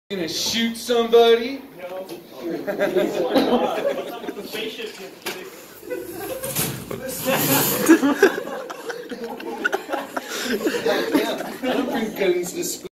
Another Day in Woodshop
Mr. Siks discovers lewd drawings on his smartboard...